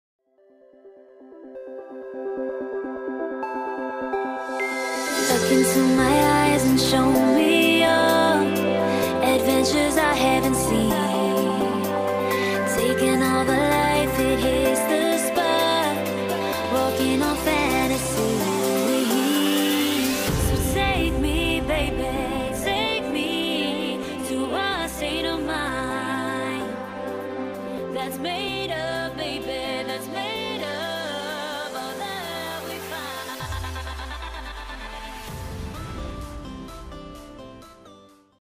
Lo Fu Tau ³⁶⁰
My brother sent me this 360 view on top of a mountain named Lo Fu Tao and my love of mountains decided to do some wizardry on post with gopro app. I'm amazed on how civilization in the concrete jungle competes with the unstable nature on that overlooking scenery,,,

btw, a big shout out sa mga Kaibigan natin dyan sa HK